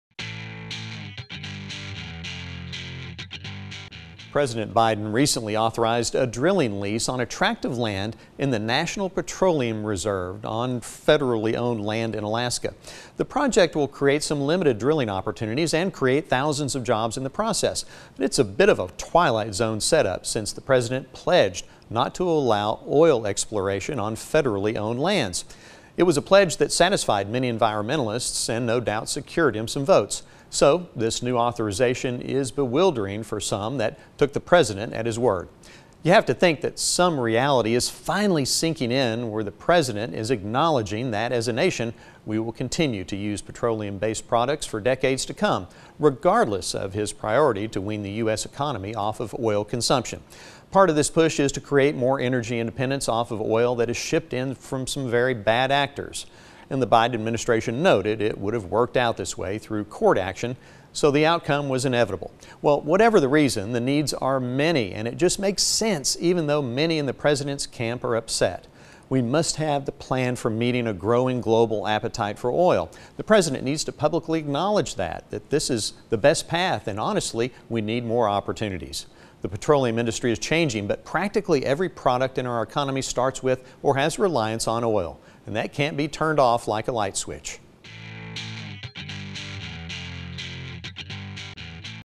A Better East Texas: Oil drilling
President Biden recently authorized a drilling lease on a tract of land in the National Petroleum Reserve on federally owned land in Alaska.